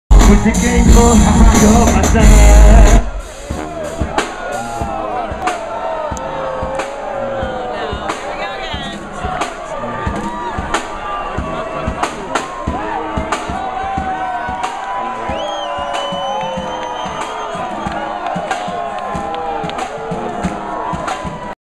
The Roots Keep It Going At Summit At Sea.MOV
The Roots rocked a Caribbean Cruise ship during the Summit at Sea--a 3 -ay conference / rock concert with 1,000 of the World's youngest and most disruptive entrepreneurs. During their Saturday night jam, the speakers blew a fuse but The Roots kept the party going. I was on the Summit Series ship reporting for Forbes Inc. -- enjoy the footage.